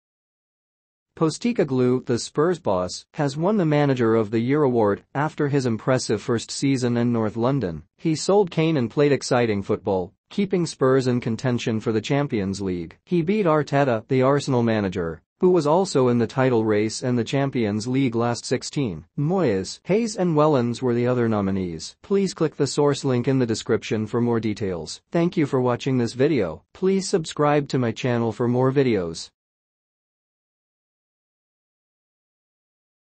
Postecoglou beats Arteta to manager of the year award
Postecoglou, the Spurs boss, has won the manager of the year award after his impressive first season in north London. He sold Kane and played exciting football, keeping Spurs in contention for the Champions League. He beat Arteta, the Arsenal manager, who was also in the title race and the Champions League last 16. Moyes, Hayes and Wellens were the other nominees.